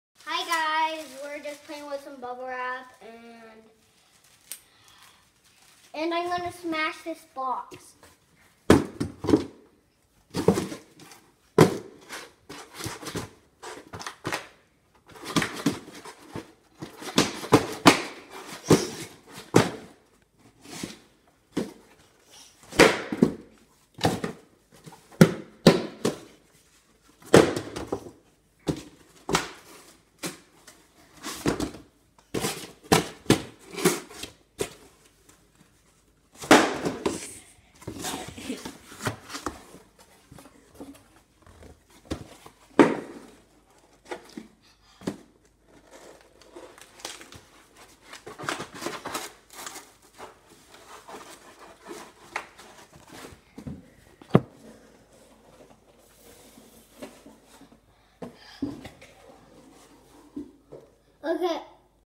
February 20, 2021
Smashing box and showing you my new ran cup bowl and plate!